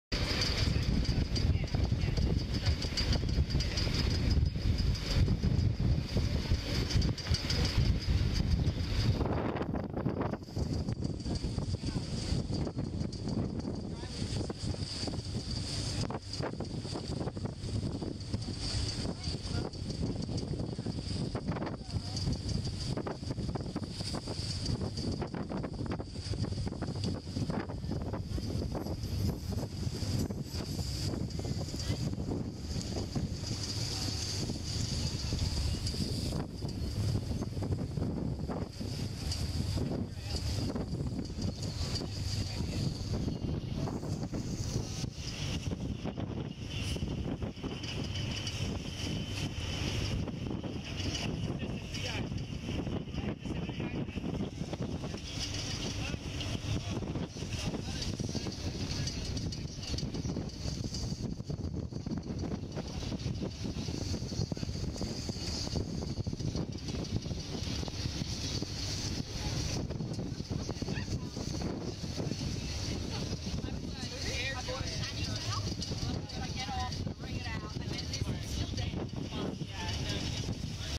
Returning to the boat (Pro Dive, Cairns)
Great Barrier Reef, Australia, December 2018